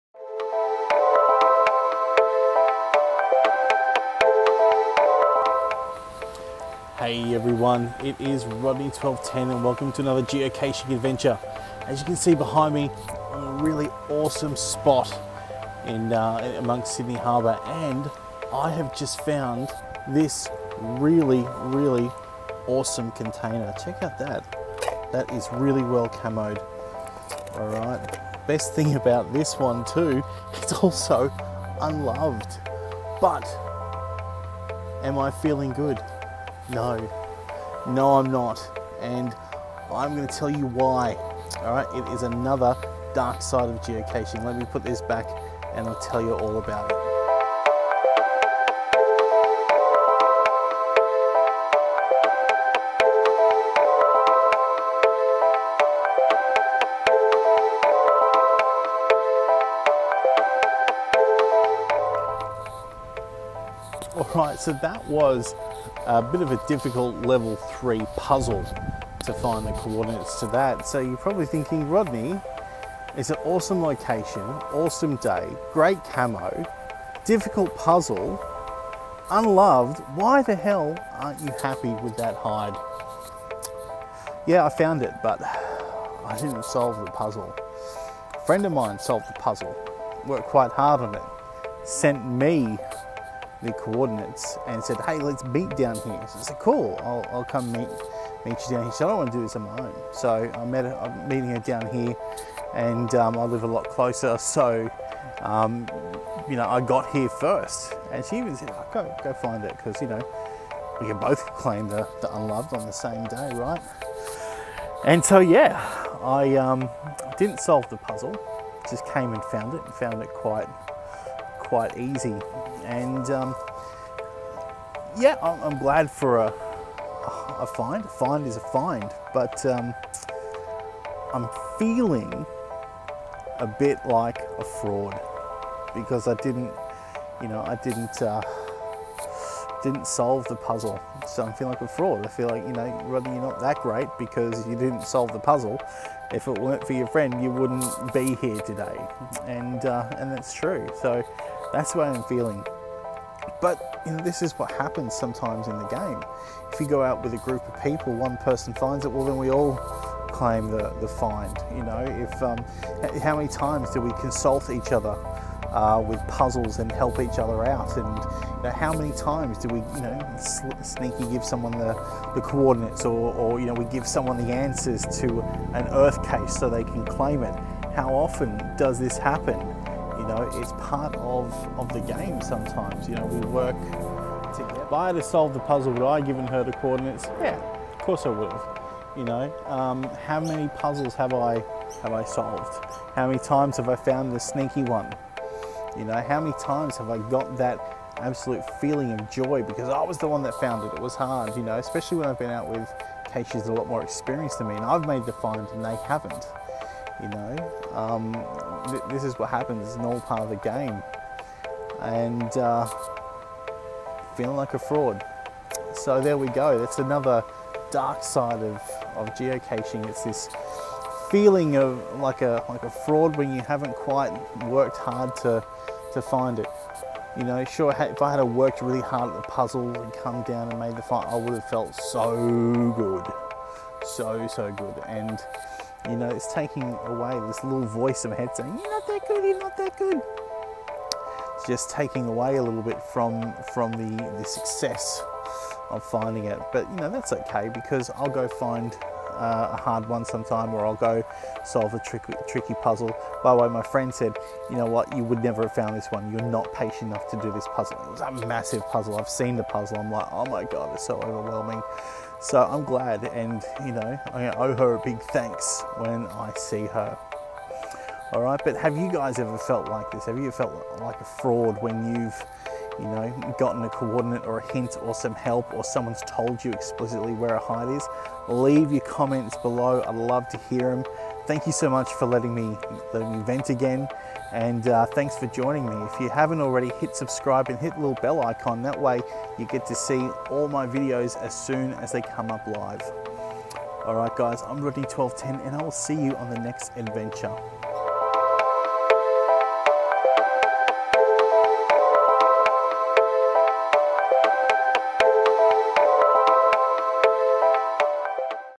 The Dark Side Of Geocaching: Fraud
Sometimes we find a cache without having to do all the work for it. Sometimes we get help with puzzles, sometimes someone in our group makes the find and sometimes we’re just given the coordinates. This happened to me recently and it didn’t feel as good as working for it myself.